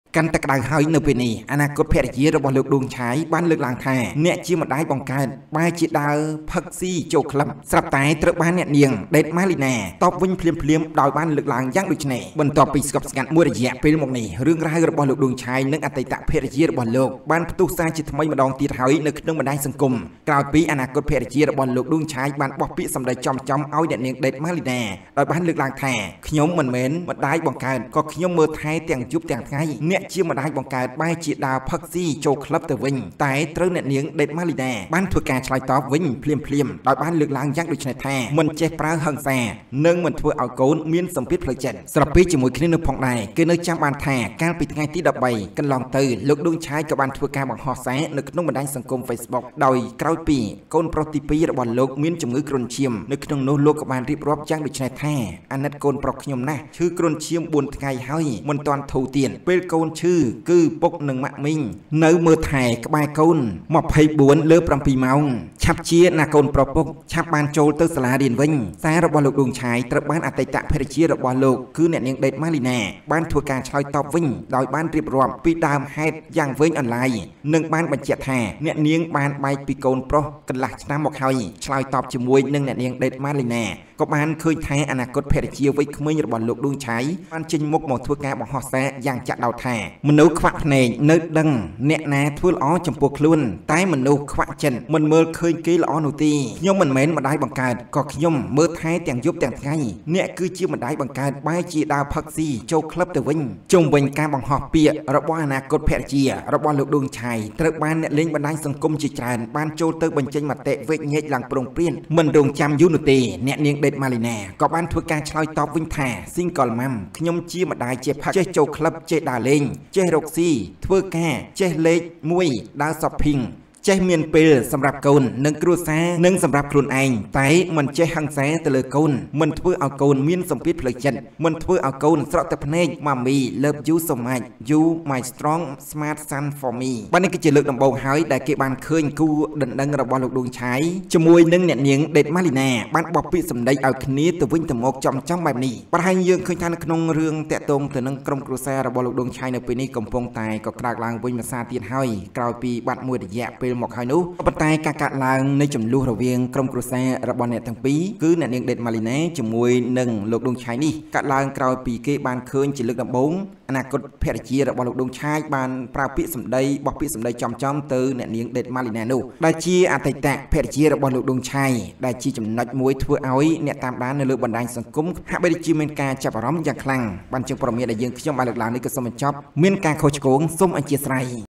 អនាគតភរិយាលោក ឌួង ឆាយ ថា«អ្នកជាម្ដាយបង្កើតបែរជាដើរផឹកស៊ីចូលក្លឹប » តែត្រូវអ្នកនាង ដេត ម៉ាលីណា តបវិញ
​ក្តៅៗ អនាគតភរិយាលោក ឌួង ឆាយ ថា«អ្នកជាម្ដាយបង្កើតបែរជាដើរផឹកស៊ីចូលក្លឹប » តែត្រូវអ្នកនាង ដេត ម៉ាលីណា តបវិញភ្លាមៗថា... , Stand Up